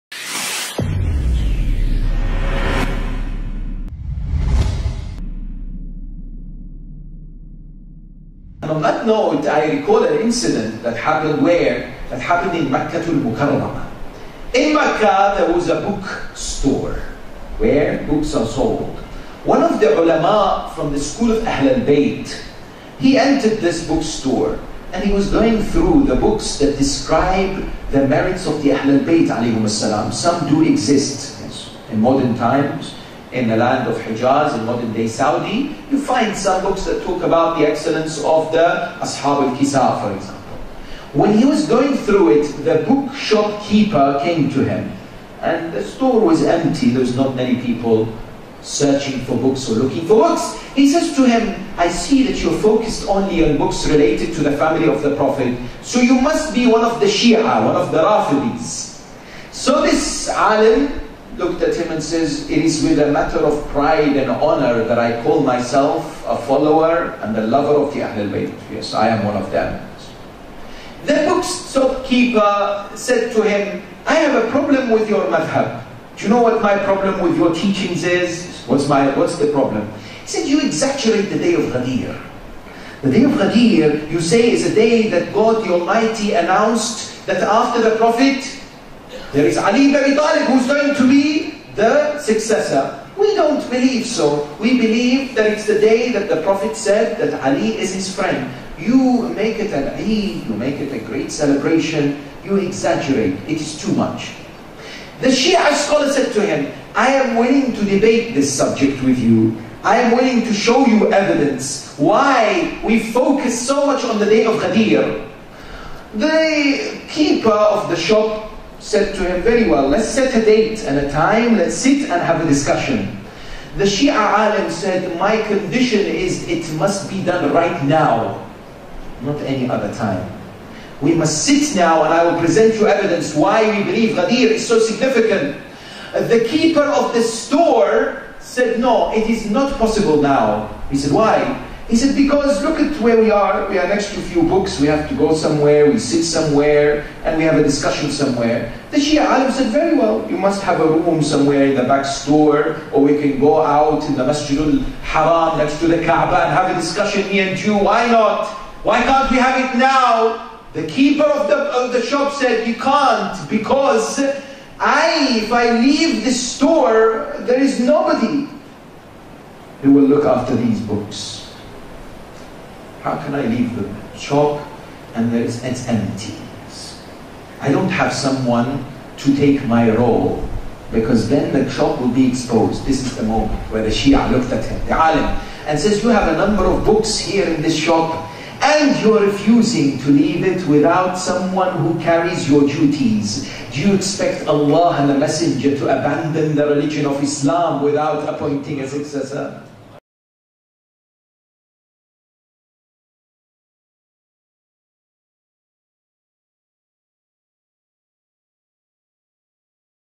Appropriate Example - Why Prophet Muhammad Appointed A Successor
A befitting example of why Prophet Muhammad appointed a successor to take care of his religion.
By Sheikh Mohammed Al-Hilli - -